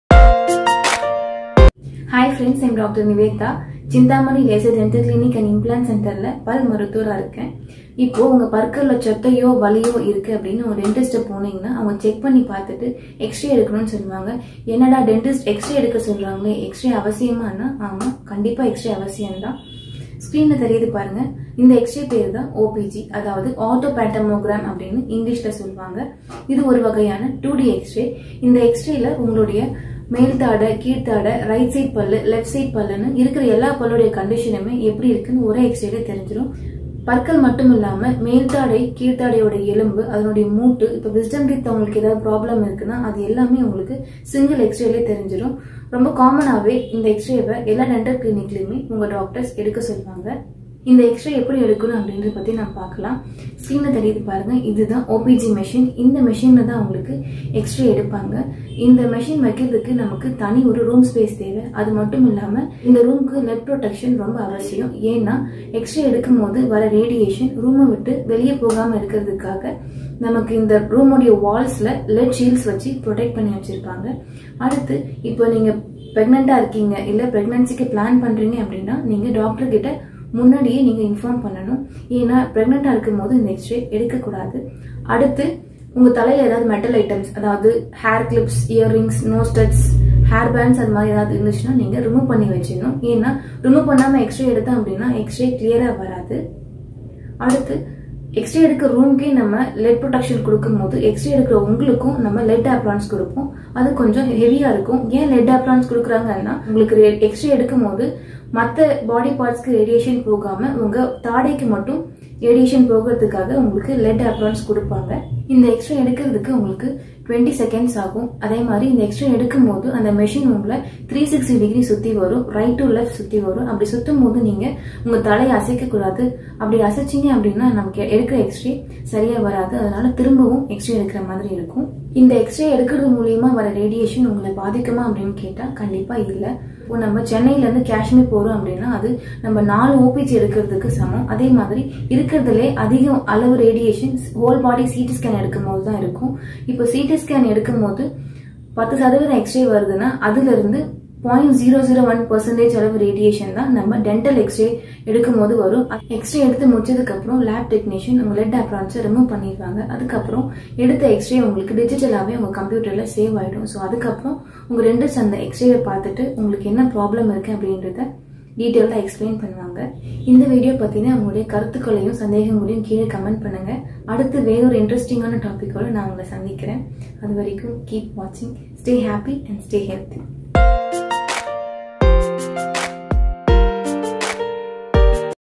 (Dental x-ray)டென்டல் எக்ஸ்ரே ஏன் தேவை? Importance of dental x-ray and how it is taken?
Importance of taking dental x-rays [opg(orthopantoogram),3D xray-CBCT(cone beam computerised tomograohy), IOPA(intra-oral periapical radiograph)] is that clearcuts perfect diagnosis and treatment plan. All the teeth and bone condition  in upper and lower jaw, joints, wisdom tooth problem, jaw fractures can be analysed in a single x-ray avoiding radiation exposure.

How dental X-rays are taken?
Why dental x-ray is taken?
What are dental x-rays?
Is dental x-ray radiation affect us ?

For further details, visit us,
Chinthamani laser dental clinic and implant centre
Iyyapanthangal, Porur
Chennai 600056